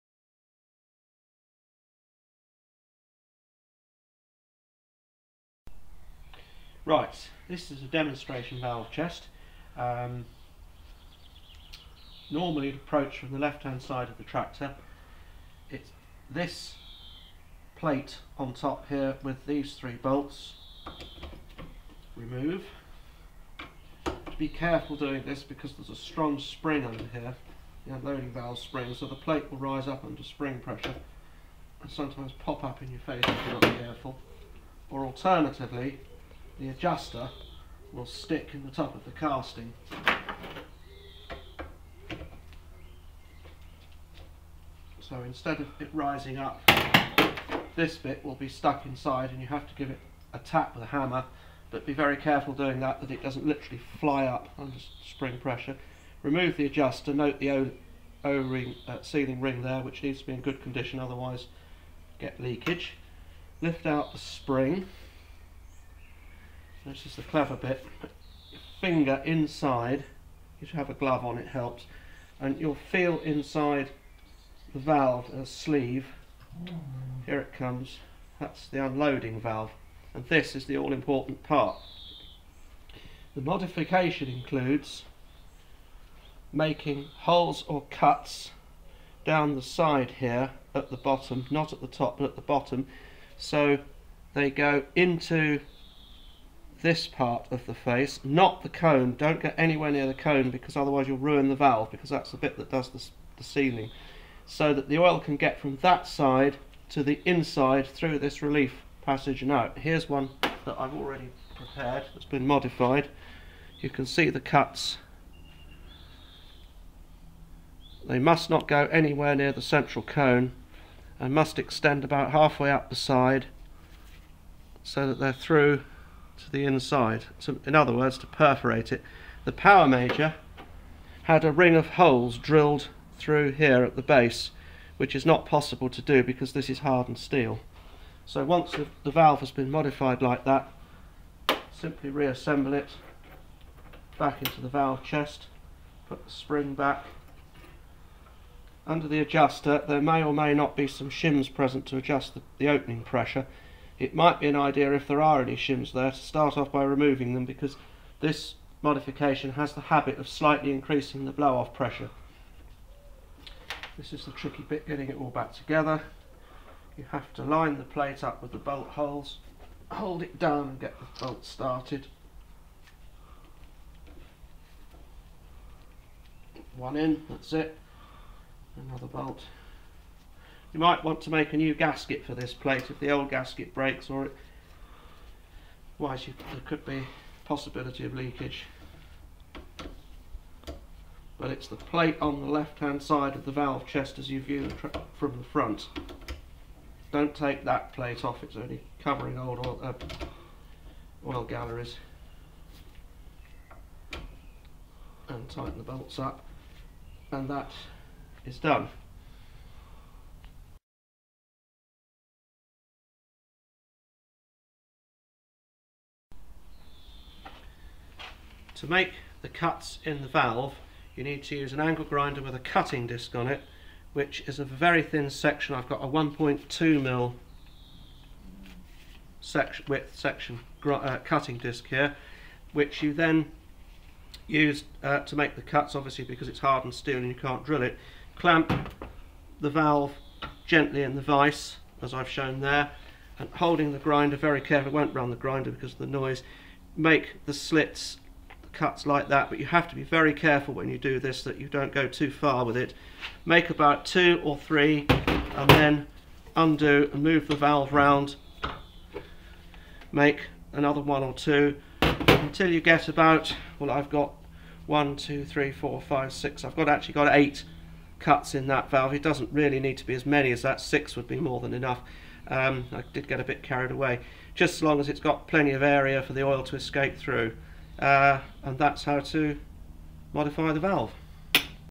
Modifying Fordson Major Diesel Unloading Valve
A short video describing how to modify the unloading valve on a Fordson Major Diesel making it more suitable for operating externally powered hydraulic equipment.